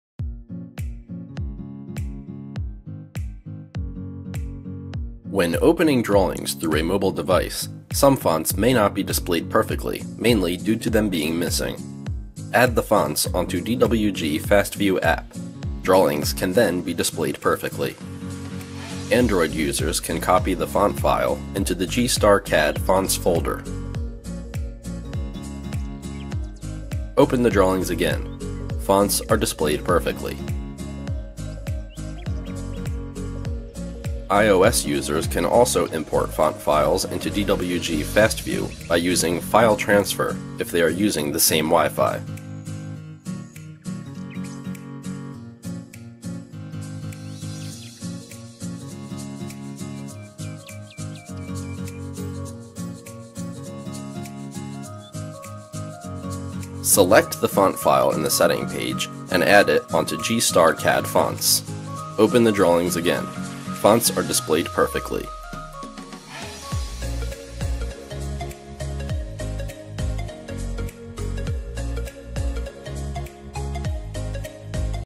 Tutorial | How to Display Fonts Perfectly in Mobile ?
When opening drawings through a mobile device, some fonts may not be displayed perfectly mainly due to them being missing.
Add the fonts folder onto DWG FastView App. Drawings can then be displayed perfectly.
This video introduces the method of importing fonts folder for Android and ios.